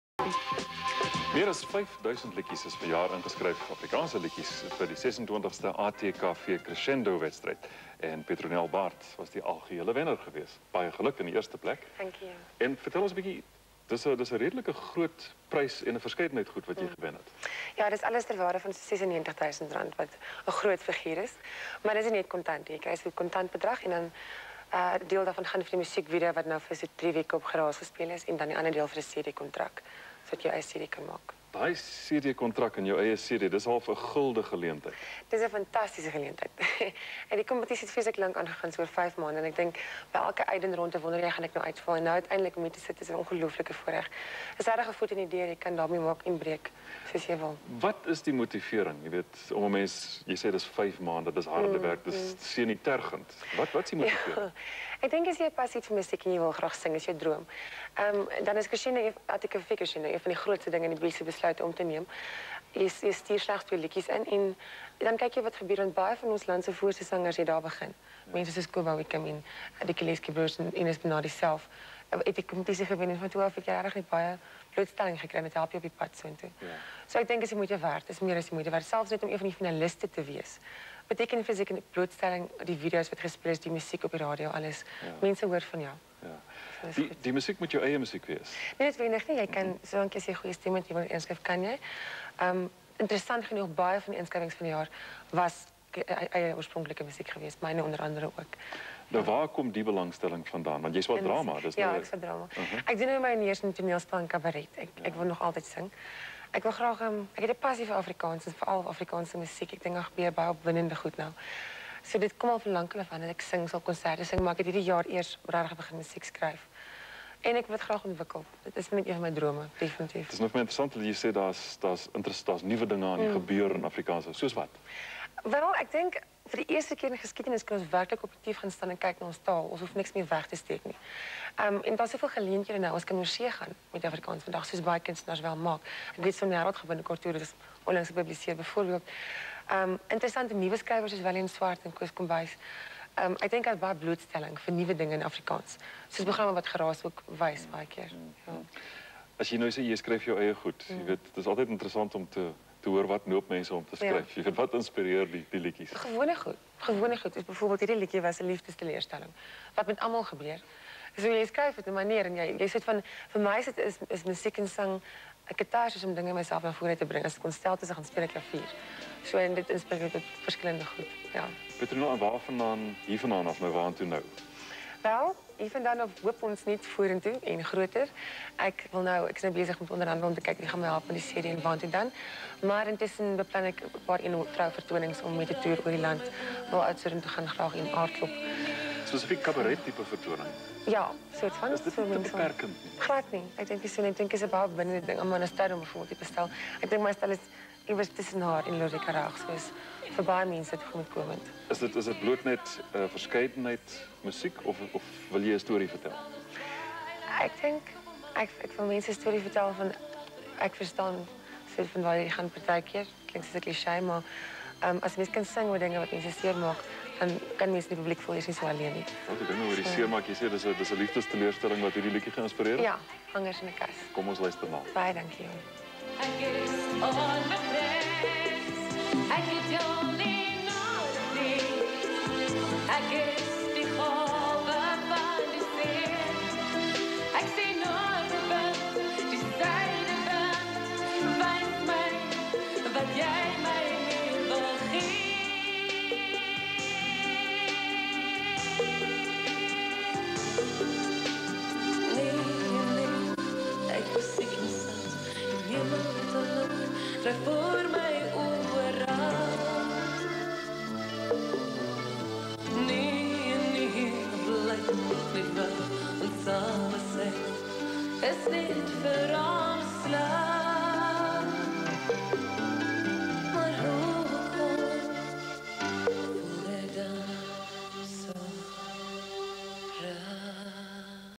LV TV Interview with Eon de Vos.
My second live TV interview in my life. ATKV Crescendo 1999